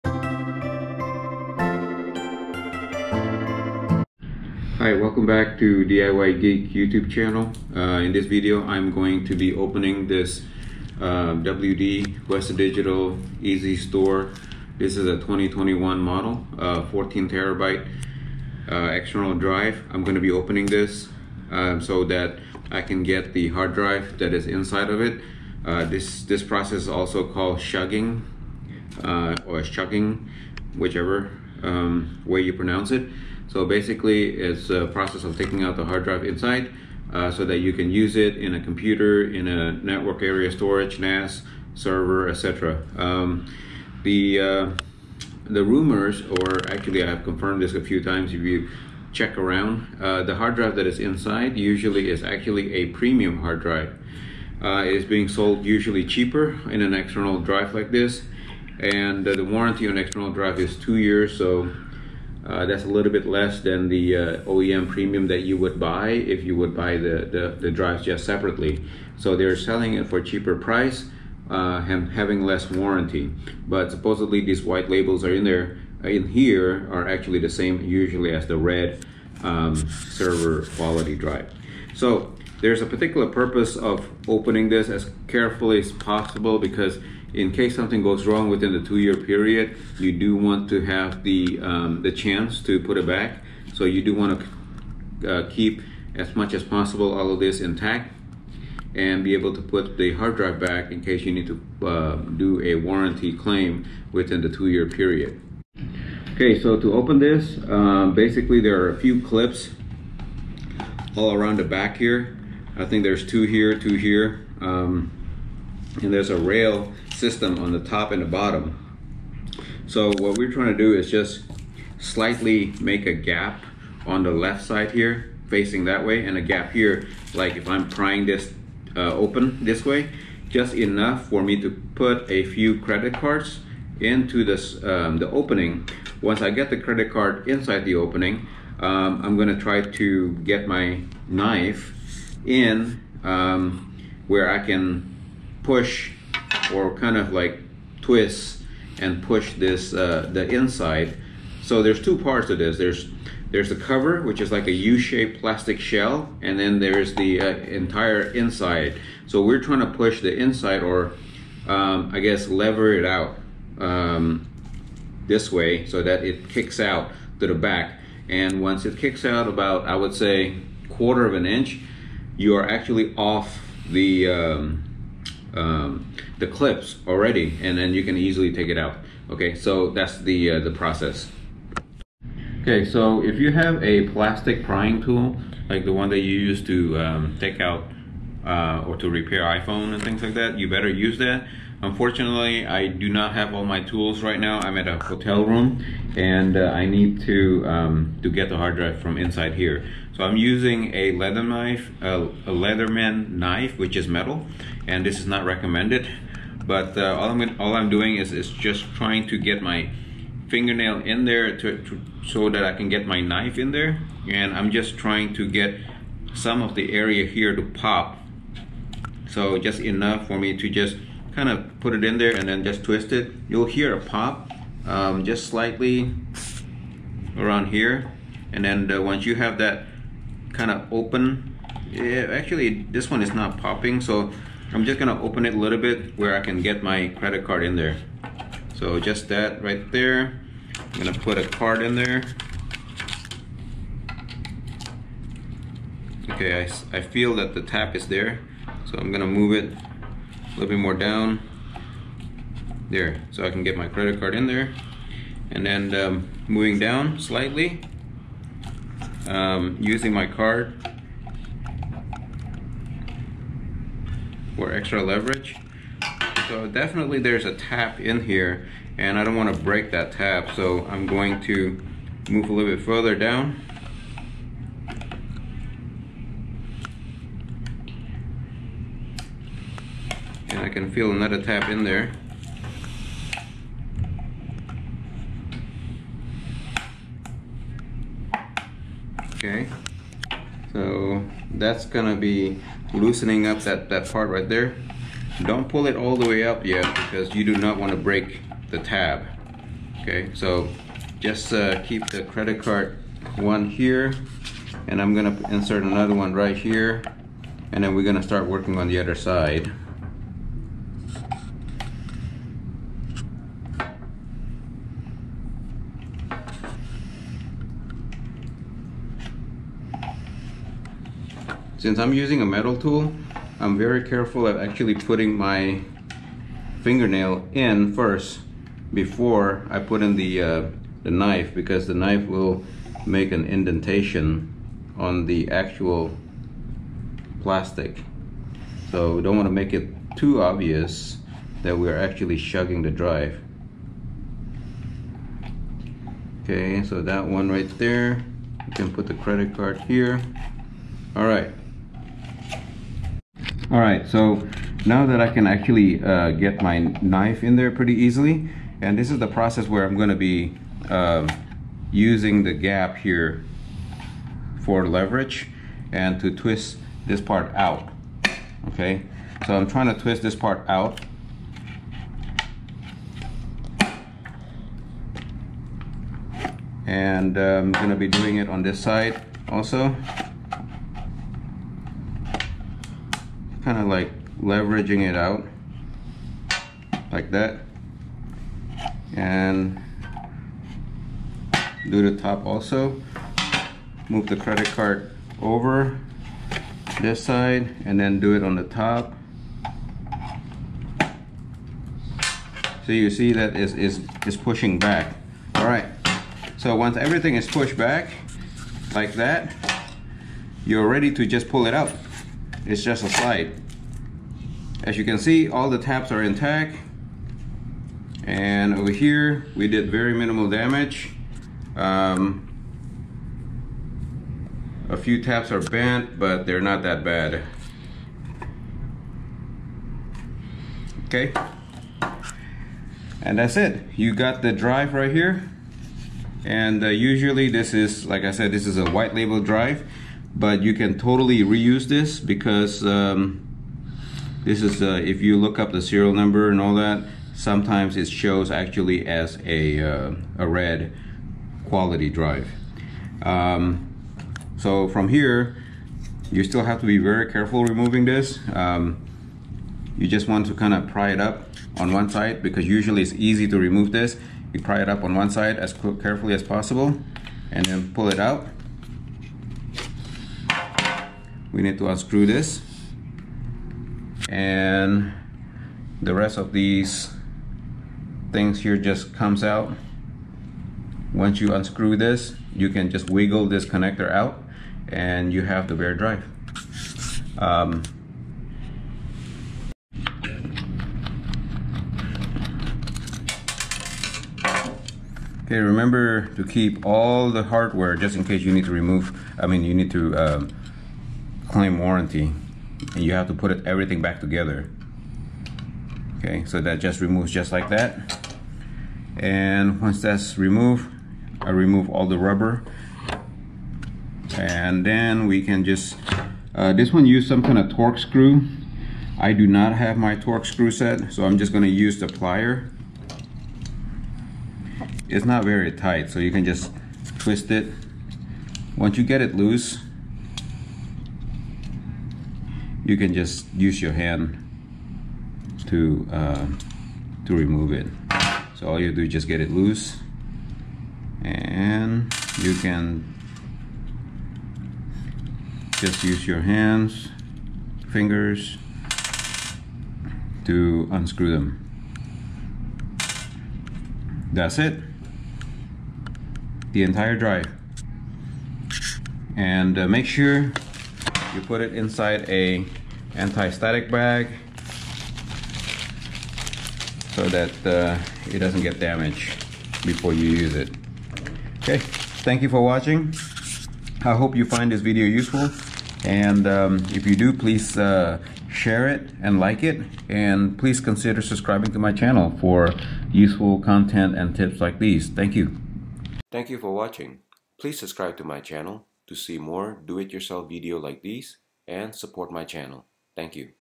REMOVING HARD DRIVE FROM WD EXTERNAL ENCLOSURE (SHUCKING HDD)
I will show you how to remove / shuck an internal hard drive from a Western Digital External enclosure with minimal damage using just a knive and several credit cards (buy prying tool from link below).